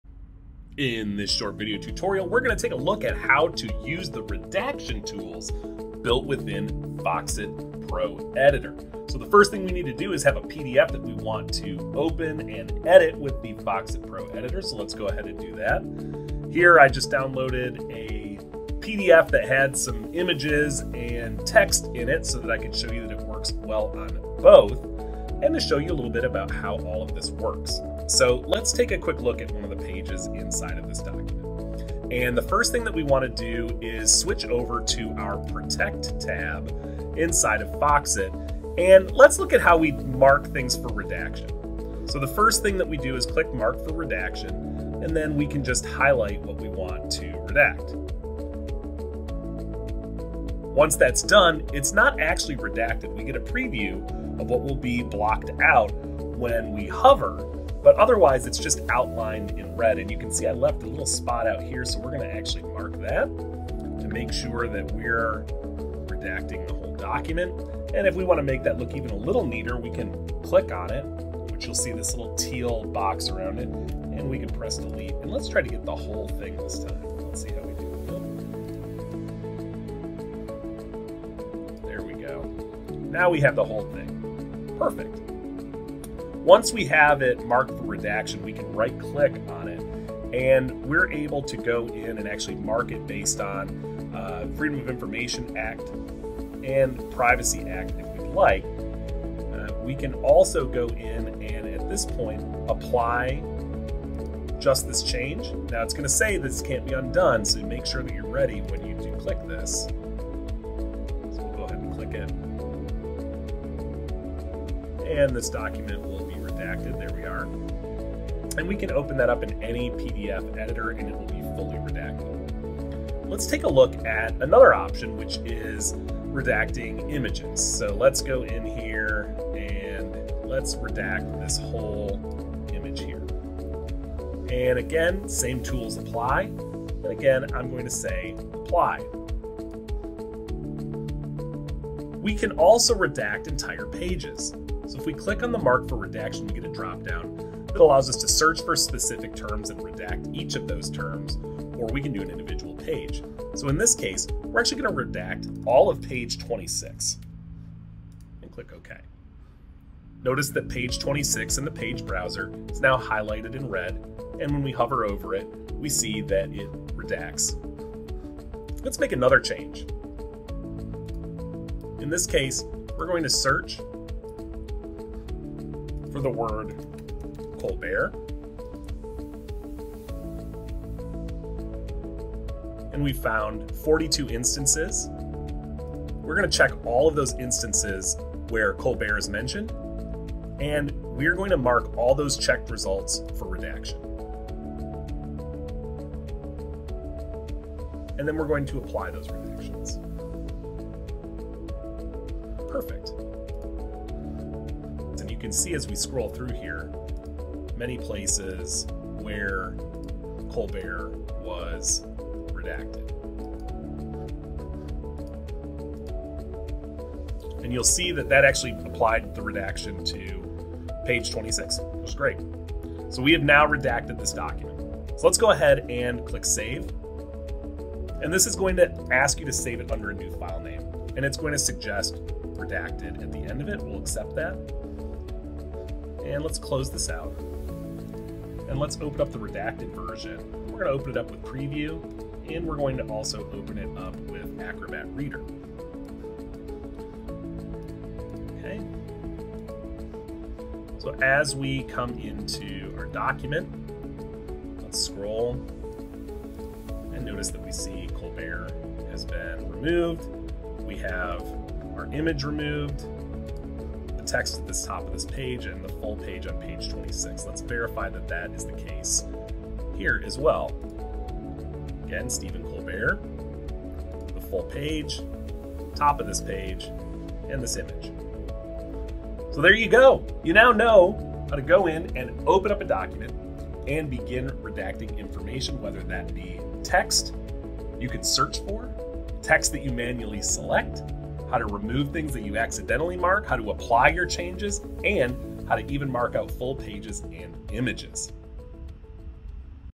Redacting Text Using Foxit Pro
This tutorial will walk you through the process of redacting text, images, and even whole pages in multiple ways using Foxit Pro!